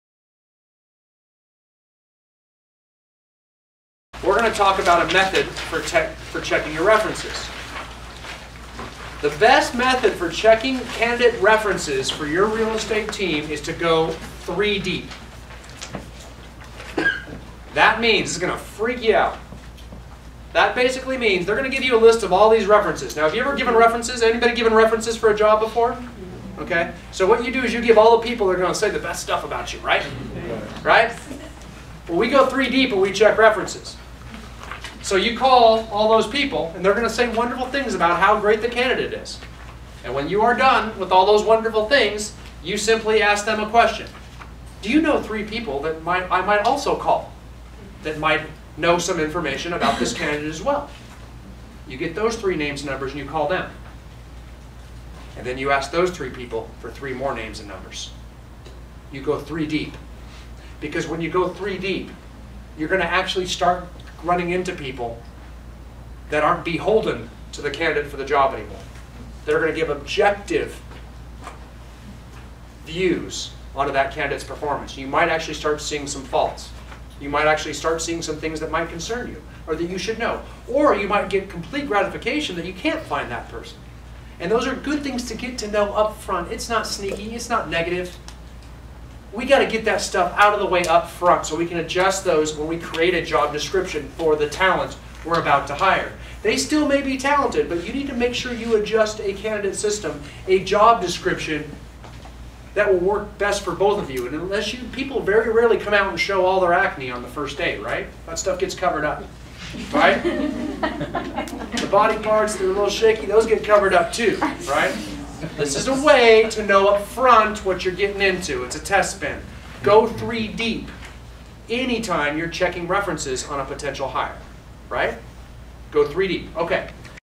Building a Real Estate Team - Go 3 Deep Checking References
- Real Estate Coach & Trainer Brian Icenhower explains the importance of "Going 3 Deep" when hiring to grow your real estate team.